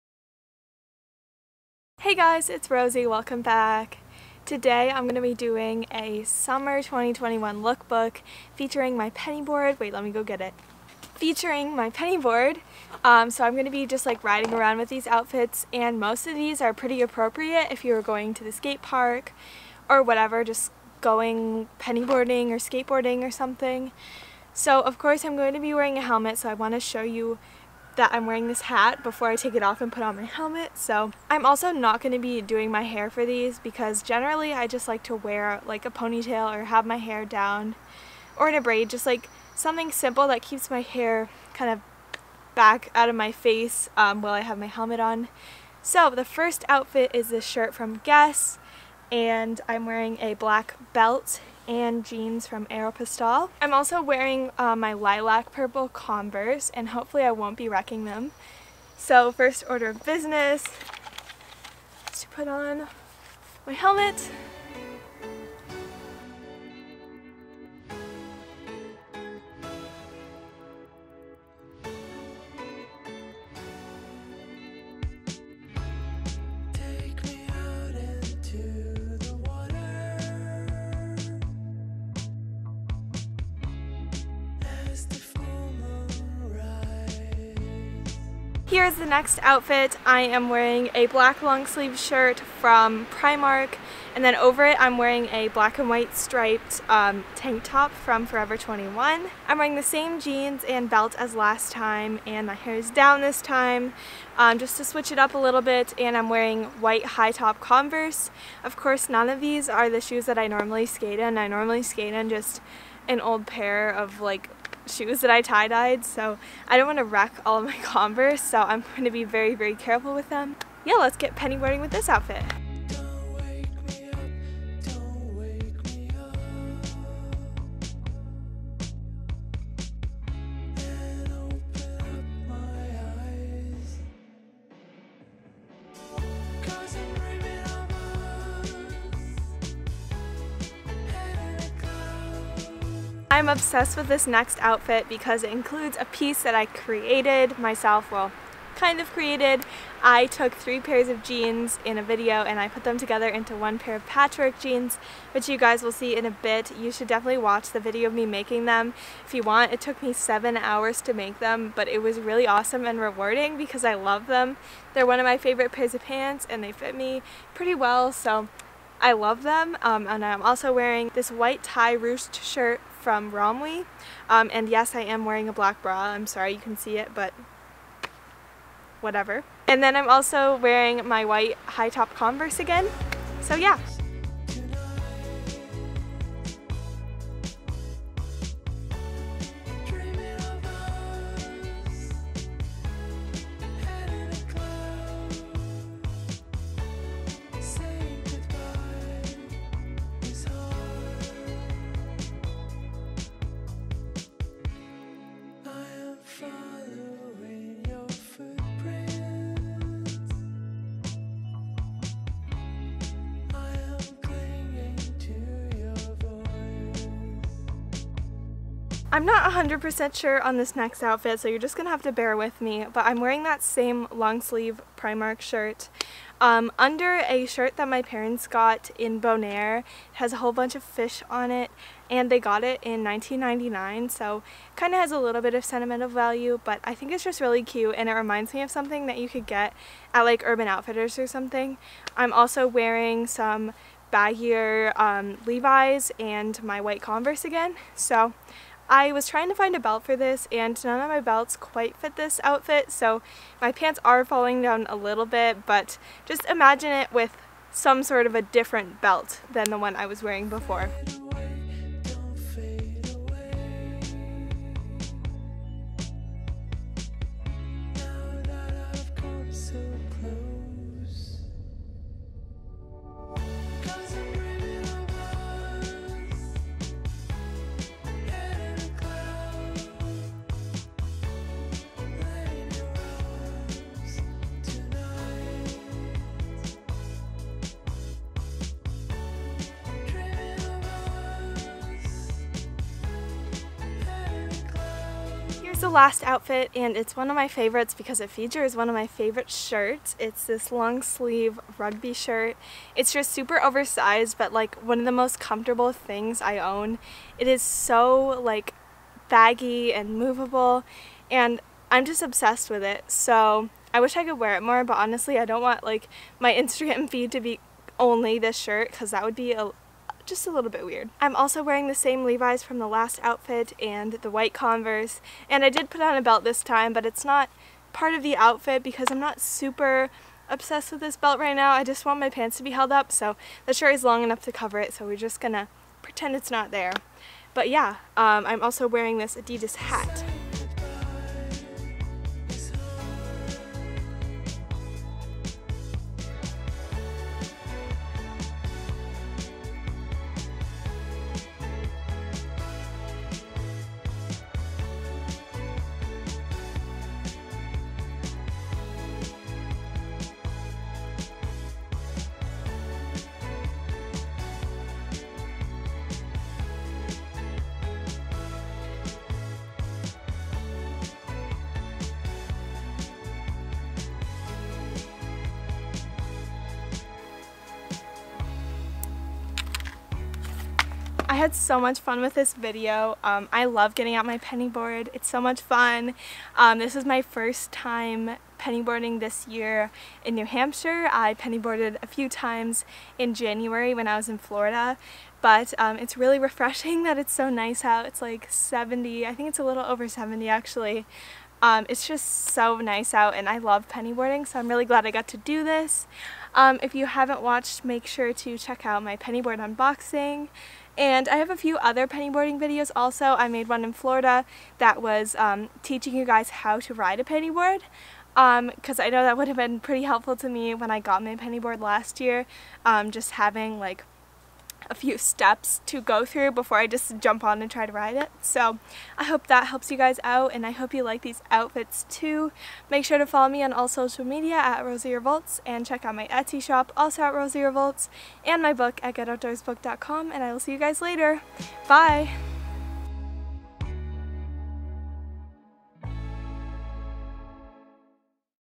Summer 2021 Penny Boarding Lookbook
Check out some of my favorite Penny boarding outfits that I will be wearing this summer!